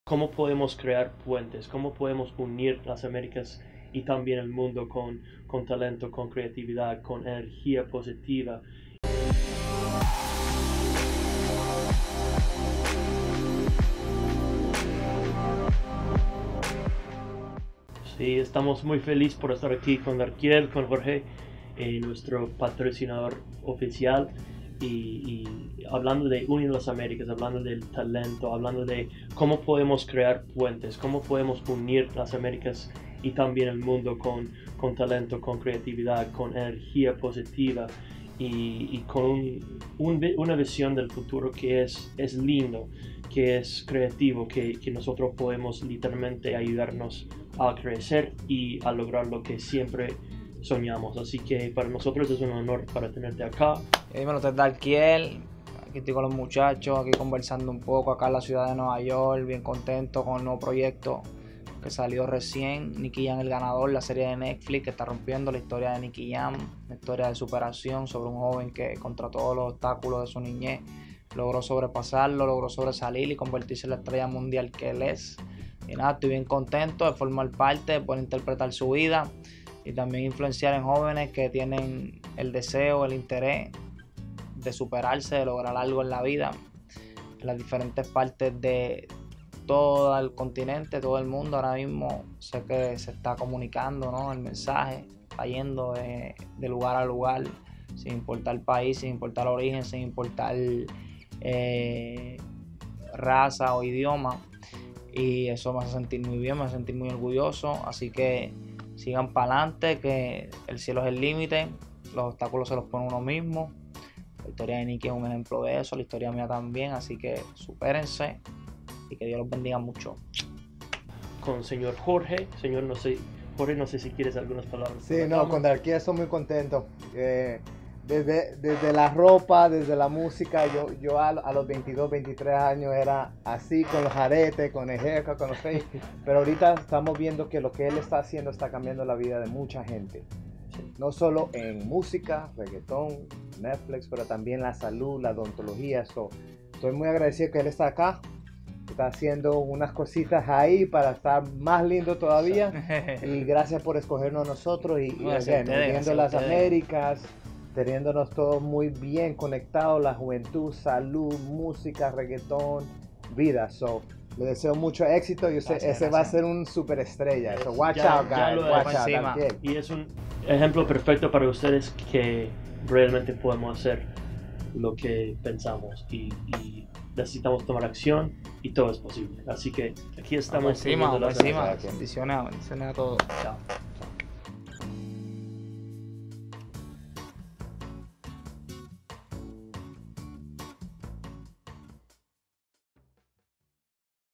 Brian Rashid, Darkiel, and Nuñez Dental Unite the Americas | BTV 165
Jorge Nuñez is one of the best entrepreneurs I know in New York City and Nuñez Dental is a pioneer for service-based business’ across the world.

Darkiel plays Nicky Jam in the new massively successful Netflix series El Ganador.

It was such a blast coming together with these two superstars to bring you this impactful message of hope, resilience, and dreams coming true for entrepreneurs all over Latin America and the rest of the world.

Can’t wait to keep building bridges with professional and cultural influencers like these two.